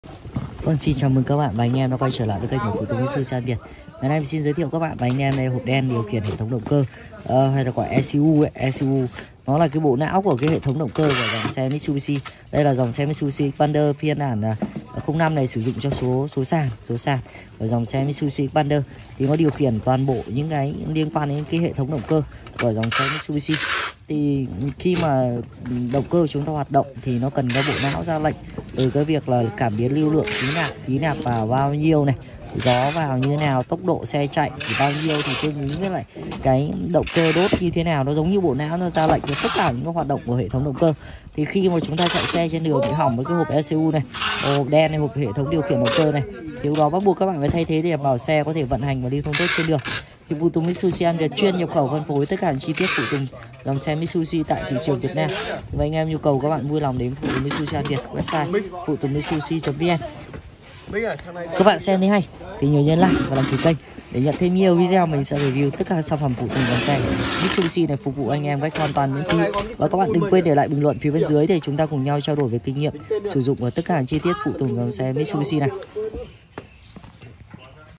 Hộp đen điều khiển động cơ xe Mitsubishi Xpander bản MT
Hộp đen ECU điều khiển hệ thống động cơ xe được sử dụng cho xe Mitsubishi Xpander số sàn MT ✅Nhập khẩu chính hãng✅Giá tốt nhất✅Cung cấp bởi Phụ tùng Mitsubishi An Việt – PhutungMitsubishi.vn